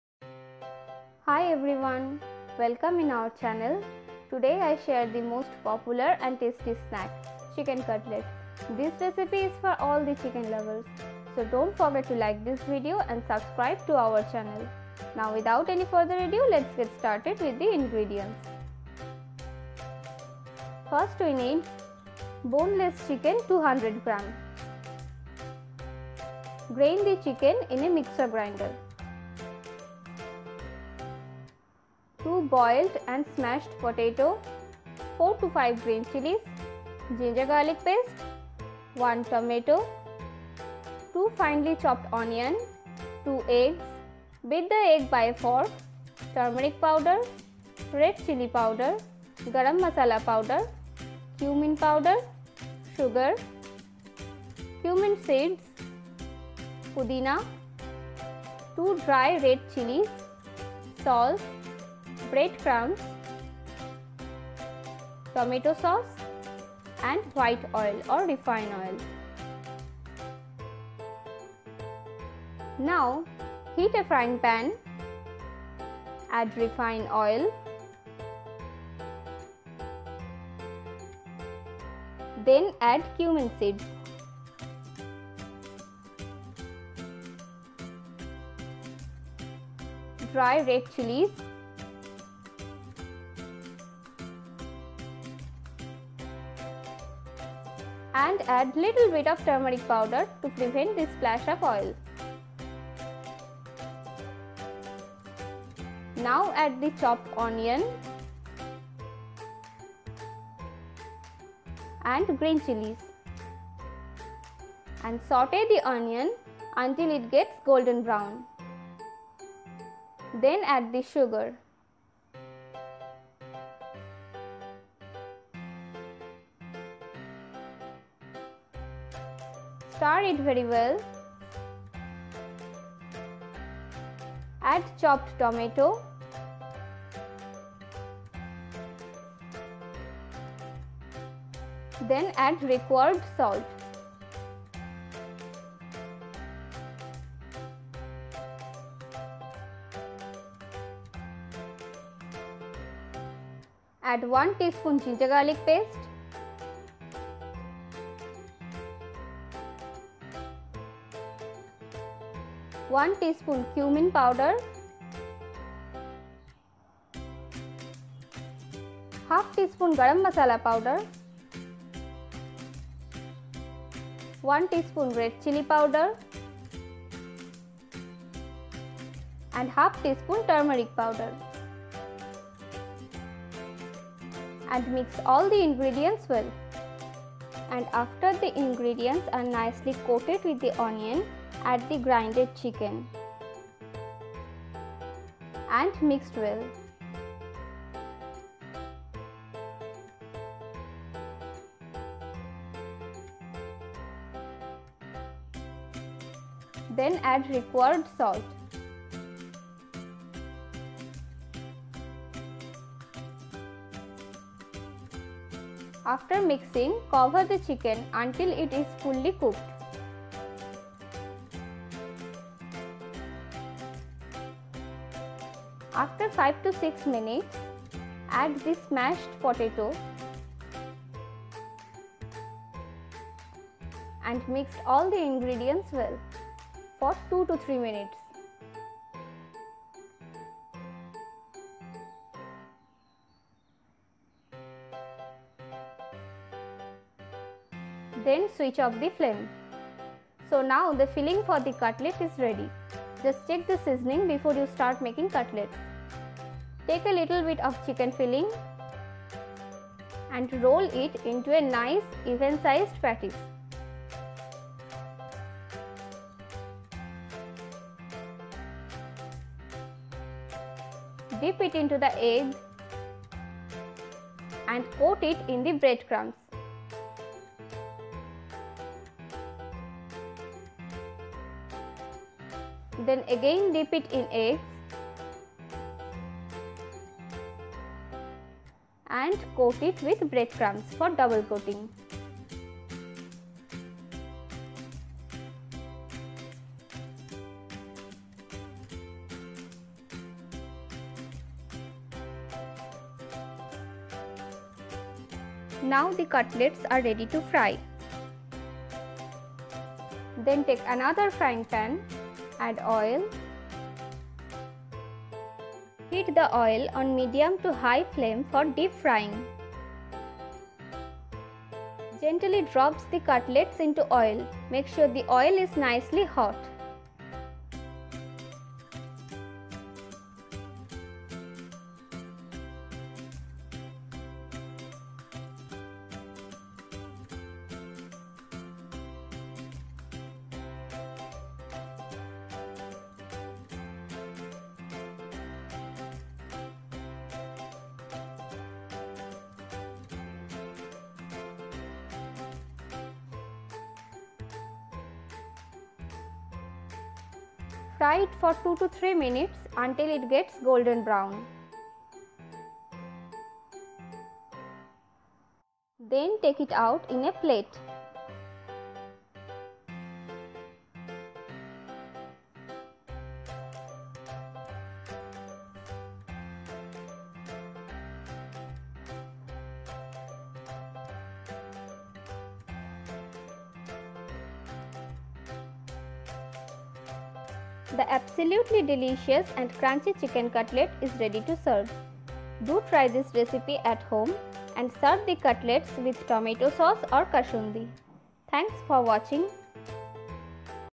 HICKEN CUTLET RECIPE EASY SNACKS RECIPE HOW TO MAKE CHICKEN CUTLET INDIAN SNACKS RECIPE
CHICKEN CUTLET RECIPE / EASY INDIAN SNACKS RECIPE  - Hi everyone! Welcome in our channel COOKING STYLE.Today I share the most popular snack "CHICKEN CUTLET".

INGREDIENTS LIST:
Chicken (Boneless) - 200gm
Potato - 2
Tomato - 1
Onion - 2
Egg - 2
Cumin seeds
Pudina leaves
Bread cramps
Tomato souce
White oil